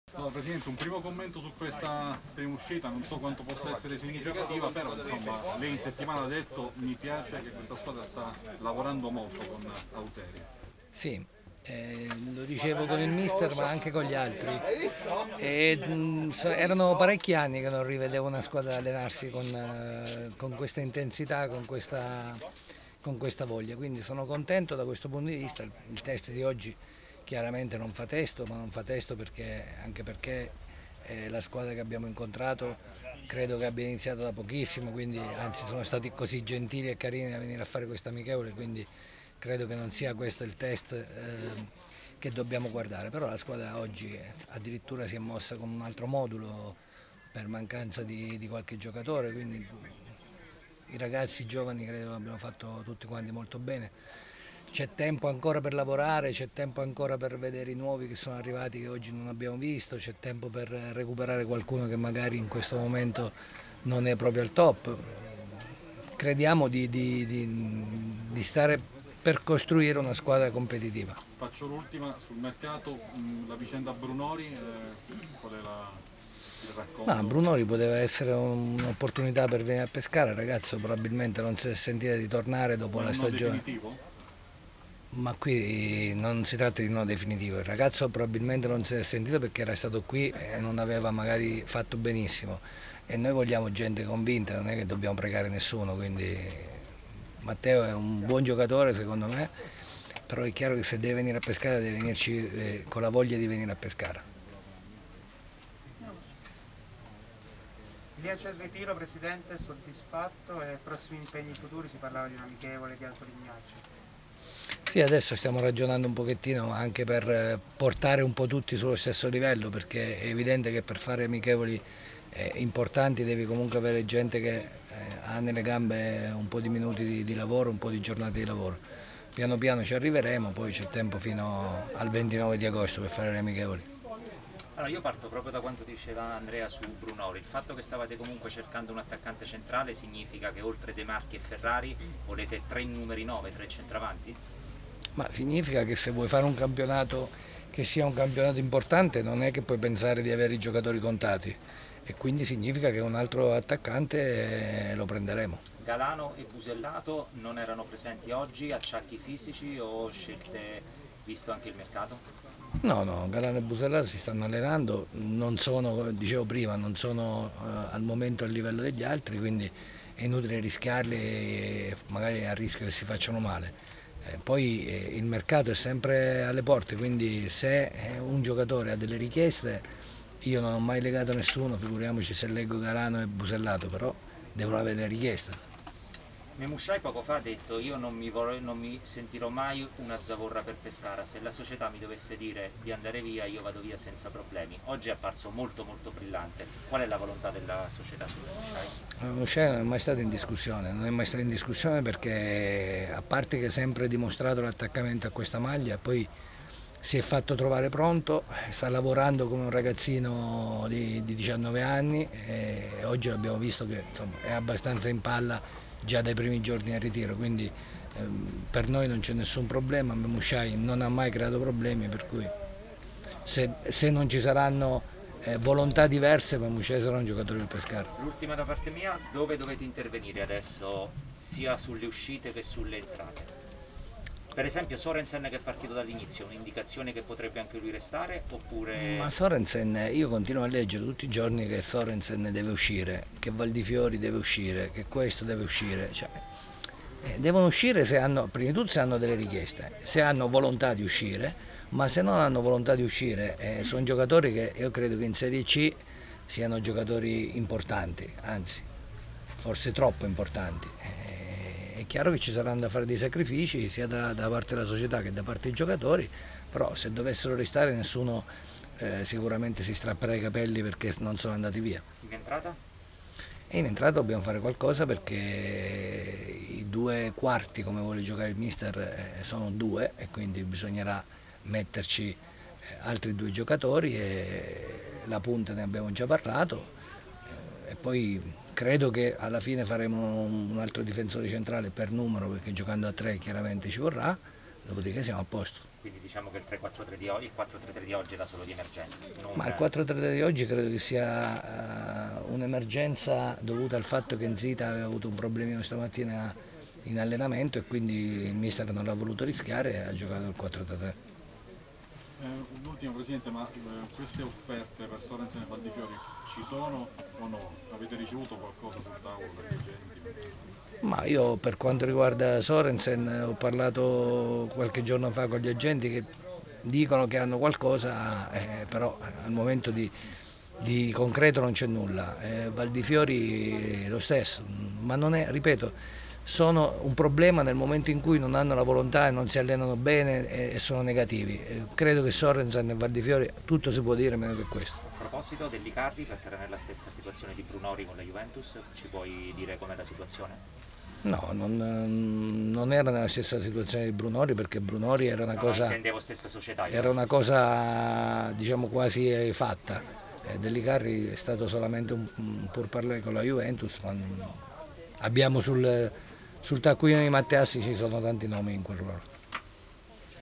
Sebastiani: "Giocatori in uscita? Servono offerte. Su Brunori..."
Conferenza del presidente Daniele Sebastiani dopo la prima amichevole stagionale del Pescara